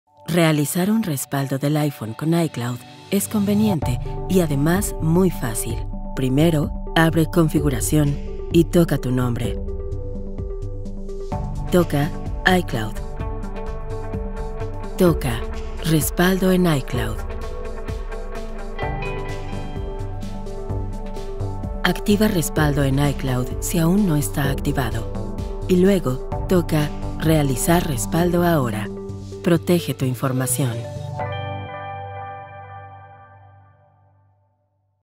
Cómo realizar un respaldo del iPhone, iPad o iPod touch con iCloud – Soporte técnico de Apple
Mantén toda la información importante del iPhone guardada de forma segura en iCloud. De ese modo, si algo le sucede al dispositivo, puedes retomar desde donde dejaste. Aquí, verás cómo usar el respaldo de iCloud.